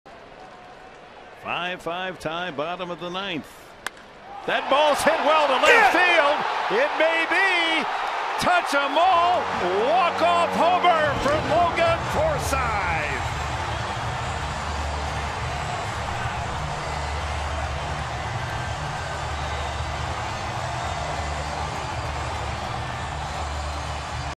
SF@SD: Forsythe crushes a dramatic walk-off home run
6/5/12: Logan Forsythe drills a dramatic walk-off home run to lead off the ninth inning, giving the Padres a 6-5 victory over the Giants